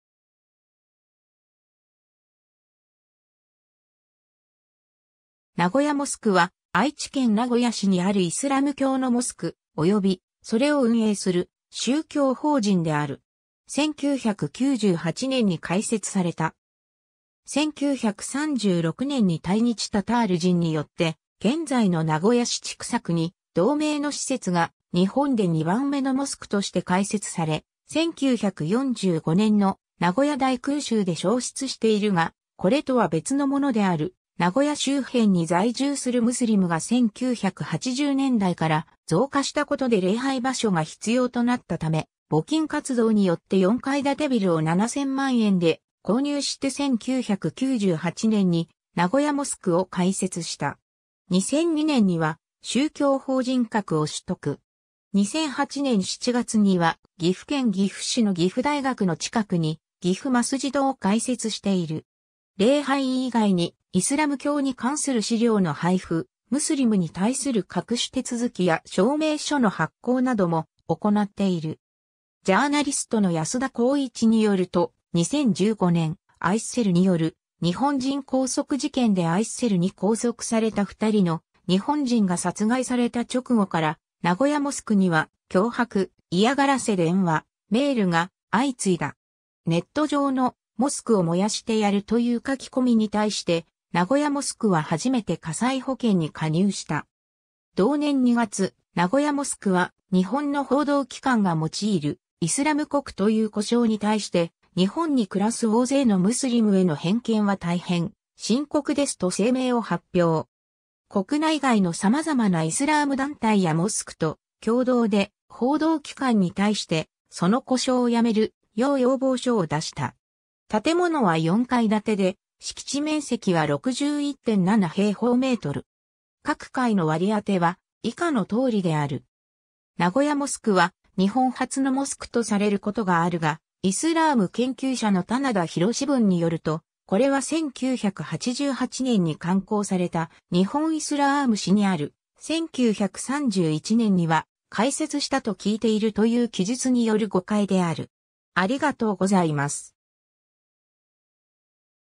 名古屋モスク
名古屋モスク（なごやモスク）は、愛知県名古屋市にあるイスラム教のモスク、およびそれを運営する宗教法人である。
1998年（平成10年）に開設された。
 1936年に滞日タタール人によって現在の名古屋市千種区に同名の施設が日本で2番目のモスクとして開設され、1945年の名古屋大空襲で焼失しているが、これとは別のものである 名古屋周辺に在住するムスリムが1980年代から増加したことで礼拝場所が必要となったため、募金活動によって4階建てビルを7000万円で購入して1998年に「名古屋モスク」を開設した。
2002年（平成14年）には宗教法人格を取得。
2008年7月には岐阜県岐阜市の岐阜大学の近くに、岐阜マスジドを開設している。
 礼拝以外にイスラム教に関する資料の配布、ムスリムに対する各種手続きや証明書の発行なども行なっている。
 ジャーナリストの安田浩一によると、2015年、ISILによる日本人拘束事件でISILに拘束された2人の日本人が殺害された直後から、名古屋モスクには脅迫・嫌がらせ電話、メールが相次いだ。
ネット上の「モスクを燃やしてやる」という書き込みに対して名古屋モスクは初めて火災保険に加入した。
同年2月、名古屋モスクは日本の報道機関が用いる「イスラム国」という呼称に対して「日本に暮らす大勢のムスリムへの偏見は大変深刻です」と声明を発表。
国内外の様々なイスラーム団体やモスクと共同で、報道機関に対してその呼称をやめるよう要望書を出した。
 建物は4階建てで、敷地面積は61.7m2。
各階の割り当ては、以下の通りである。
 ^ 名古屋モスクは日本初のモスクとされることがあるが、イスラーム研究者の店田廣文によると、これは1988年に刊行された『日本イスラーム史』にある「1931年には開設したと聞いている」という記述による誤解である。